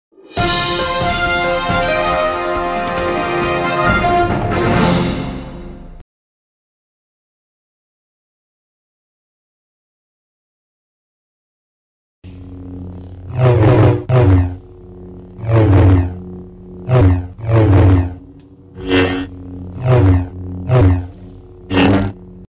Lightsaber Test
A crude test of a totally free lightsaber technique!